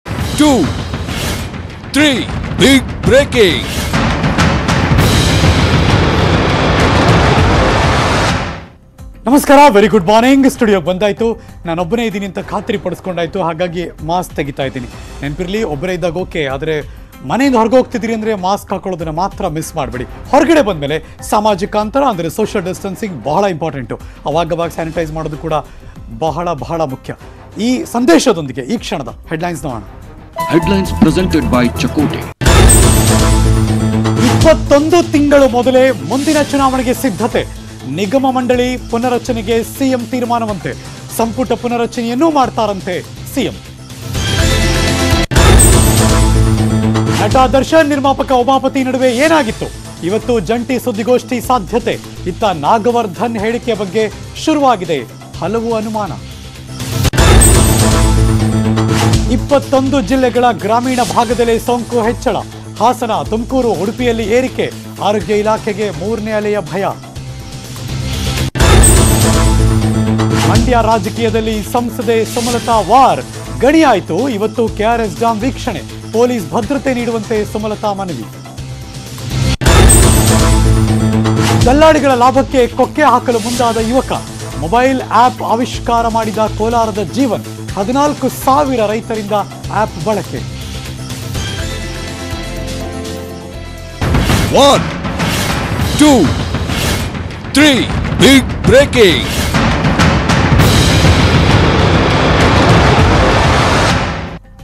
Kannada News Headlines | ಮುಂದಿನ ಚುನಾವಣೆಗೆ BSY ಸಿದ್ಧತೆ; ನಿಗಮ ಮಂಡಳಿ ಪುನರ್ ರಚನೆ!
Kannada News Headlines | ಮುಂದಿನ ಚುನಾವಣೆಗೆ BS Yediyurappa ಸಿದ್ಧತೆ ಮಾಡಿಕೊಳ್ಳುತ್ತಿದ್ದಾರೆ. ನಿಗಮ ಮಂಡಳಿ ಪುನರ್ ರಚನೆಗೆ ಸಿಎಂ ಮುಂದಾಗಿದ್ದಾರೆ.